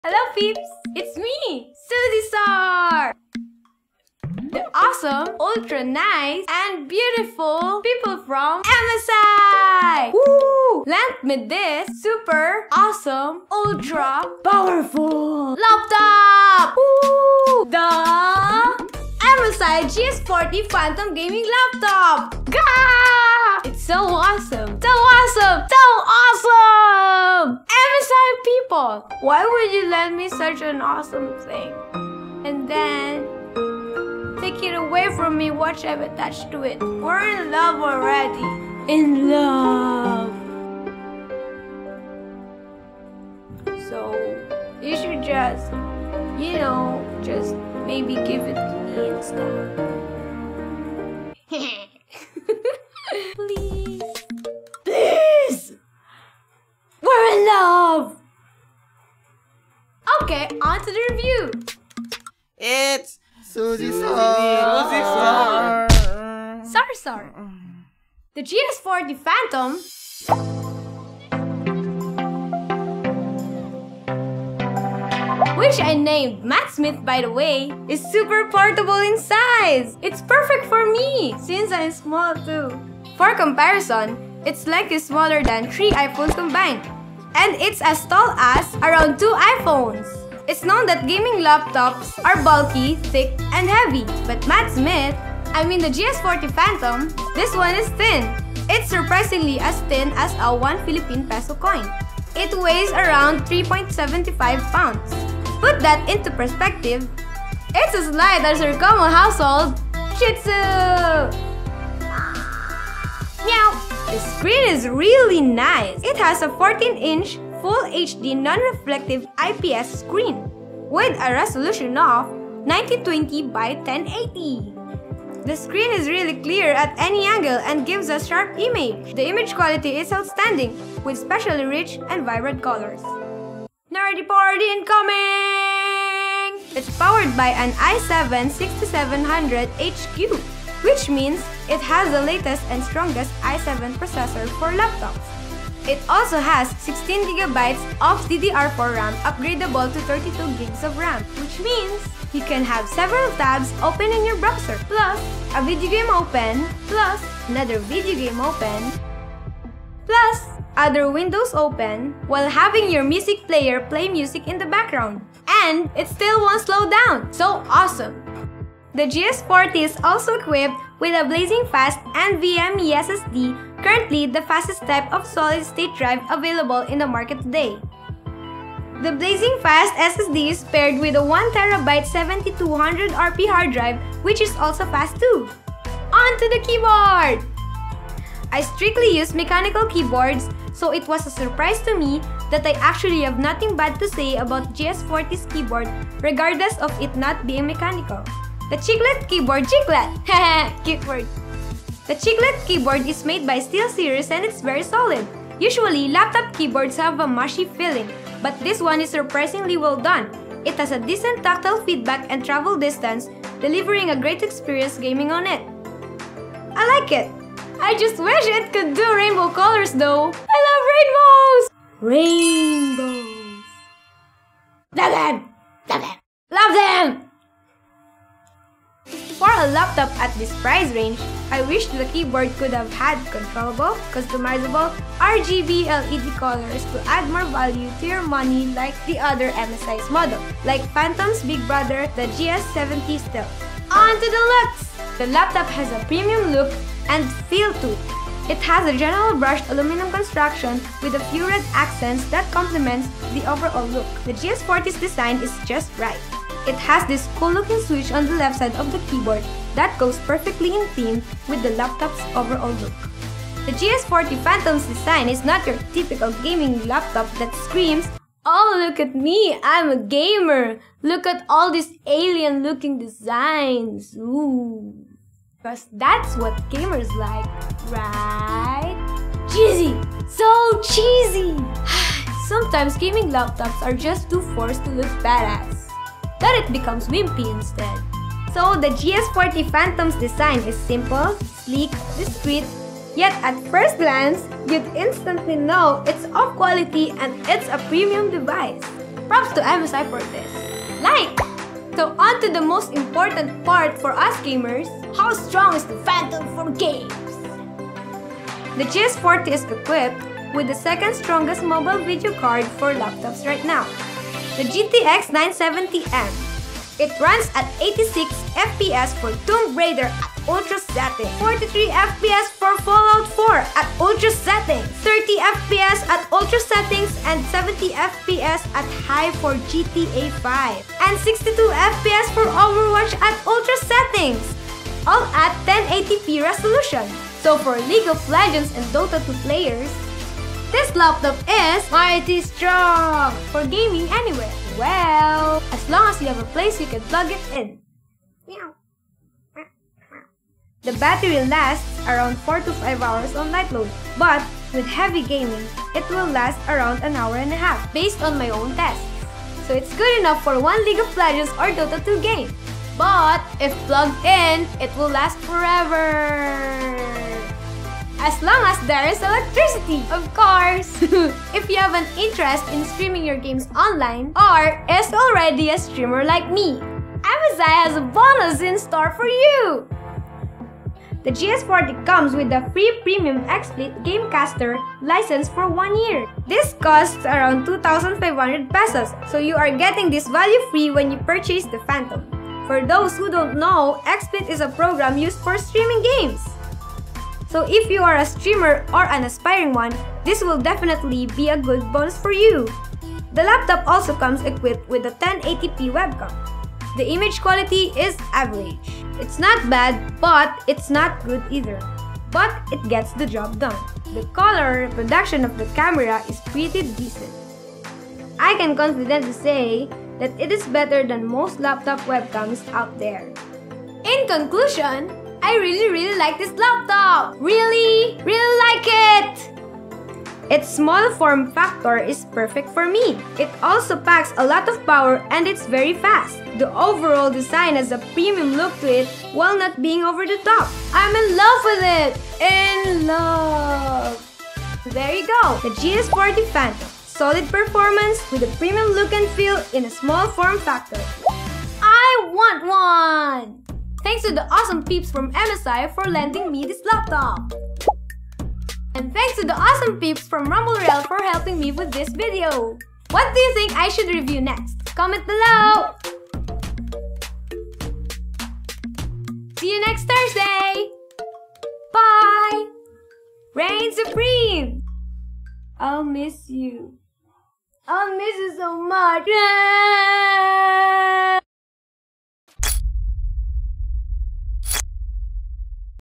A gaming Laptop as light as a SHITZU & as thin as a COIN! - MSI GS40 Phantom Review
Suzzysaur reviews the light and thin but well-tuned and efficient, MSI GS40 Phantom Gaming and streaming laptop!

Like to know more?

Check out the RUMBLE ROYALE official FB page and join our MASSIVE community and get social with other gamers!!